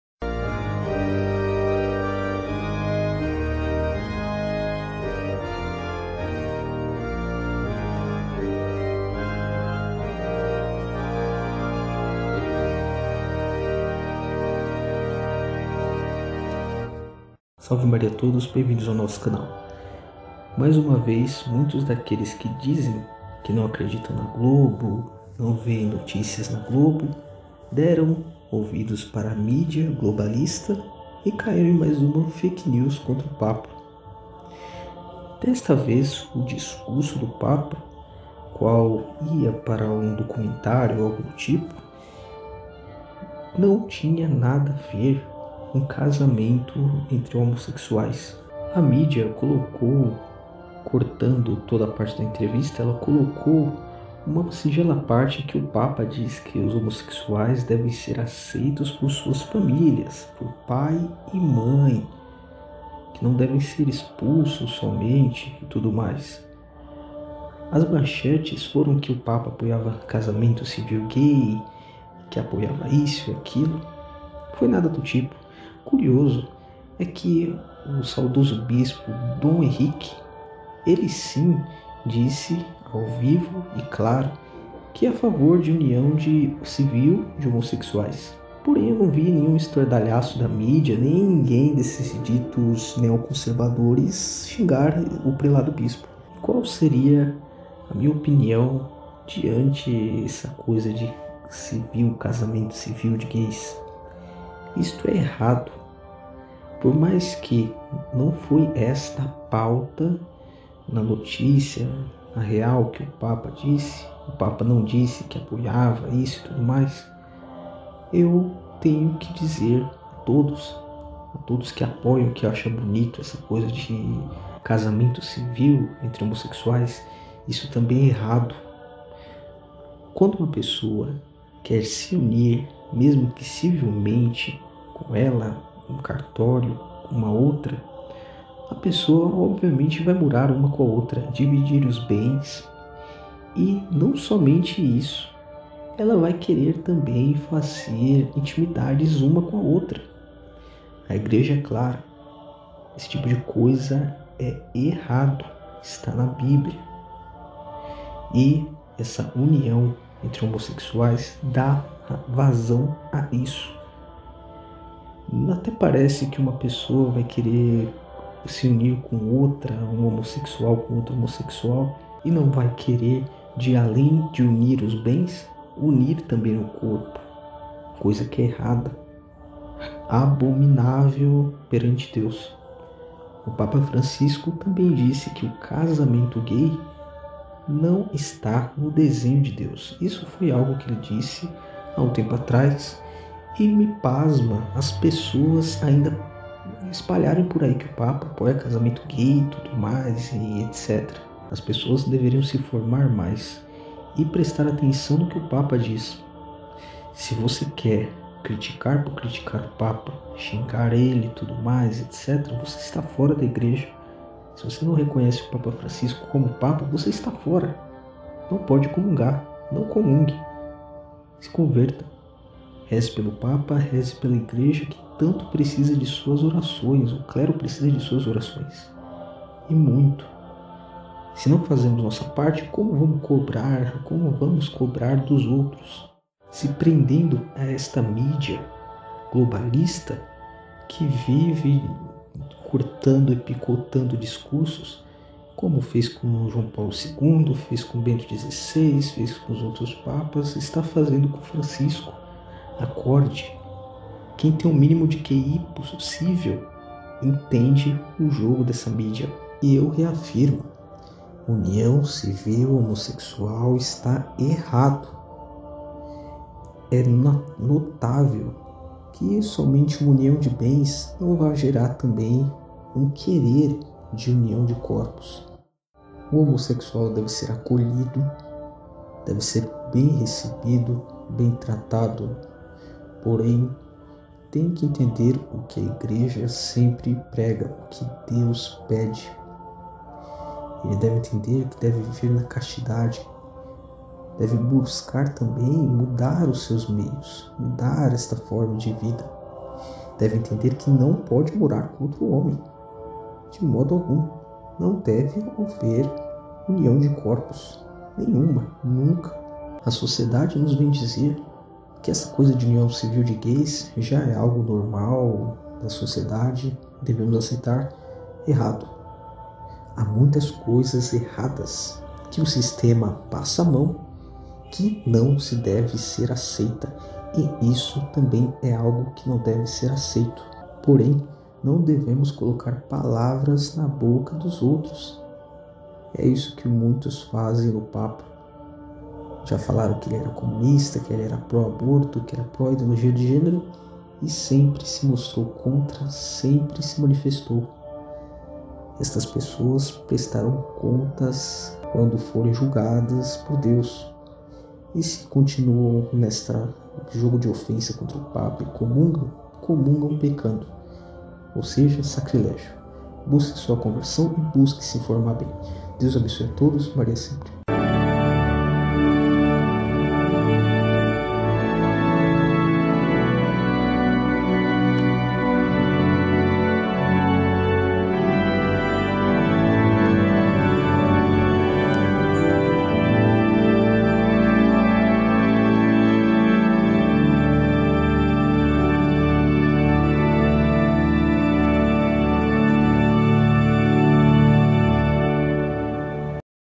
A Fake News contra o Papa e a real posição sobre a "União Civil" de homossexuais.
Você deu ouvidos a mídia? Se prestou em acreditar no que a globo e demais jornais e hereges noticiaram? Pois você está precisando de rezar mais, lavar uma louça e se informar em FONTES LEGÍTIMAS! Olha a tamanha baboseira que o laicato se meteu. Sinal que estamos distantes de ter um digno laicato para defender a Igreja e evangelizar. Ainda bem que a Igreja não depende de vocês, cismaticos! Se dependesse de vocês, seria mais uma seita de esquina e garagem com CNPJ.

E neste mesmo vídeo deixo minha clara posição sobre isso de União Civi. Está errada. Não se deve ter isso, e tenho total DIREITO de contestar a sociedade e as autoridades sobre esta lei que abre ar a muitos erros!

Site sobre milagres eucatísticos (criado por Beato Carlo Acutis)